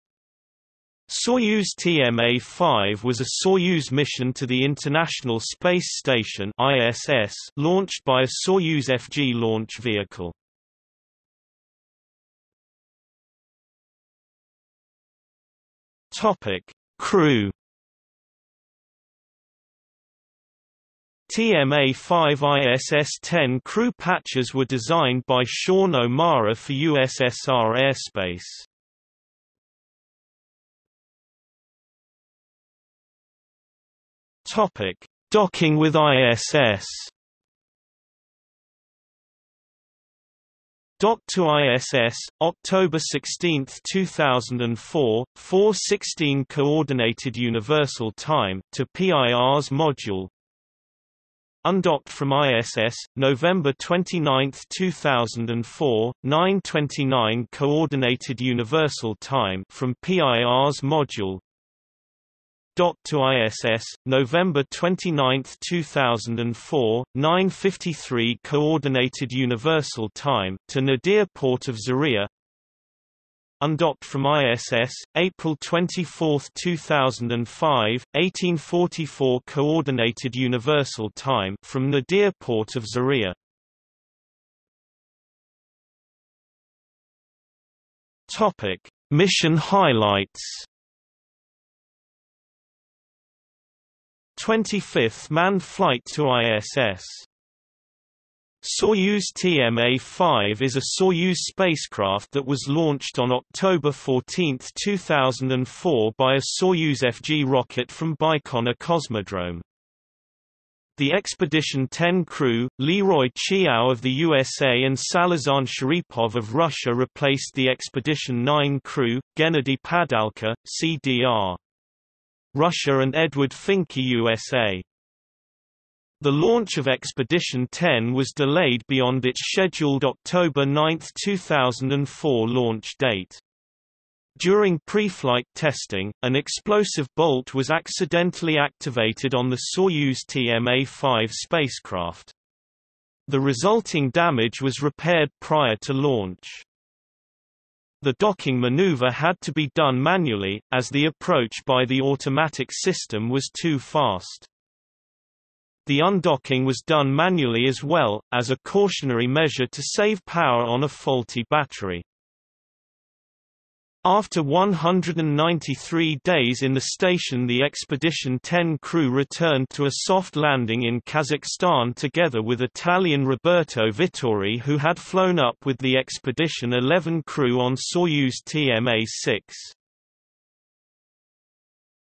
Soyuz TMA-5 | Wikipedia audio article
00:00:12 1 Crew
00:00:30 2 Docking with ISS
00:01:24 3 Mission highlights

- Socrates

SUMMARY
Soyuz TMA-5 was a Soyuz mission to the International Space Station (ISS)  launched by a Soyuz-FG launch vehicle.